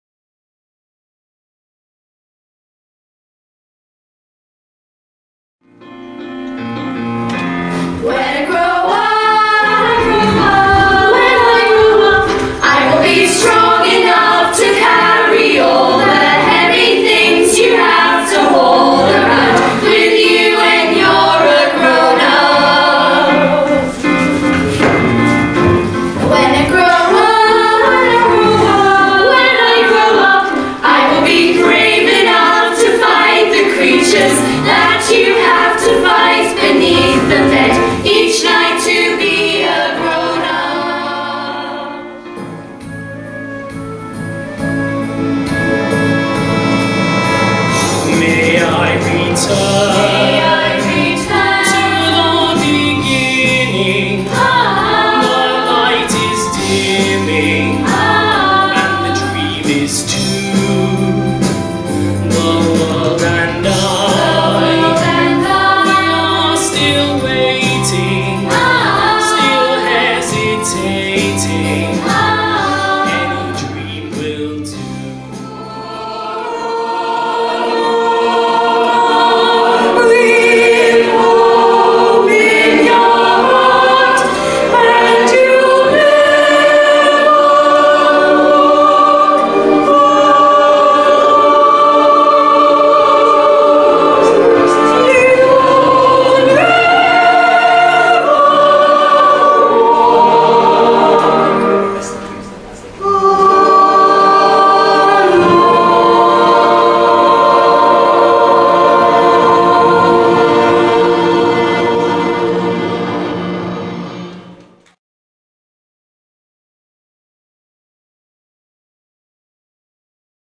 Journey of a lifetime Trailer
Preview of Berwick Operatic Society's Journey of a Lifetime, on stage at The Maltings, Berwick from 13th - 15th of October 2016.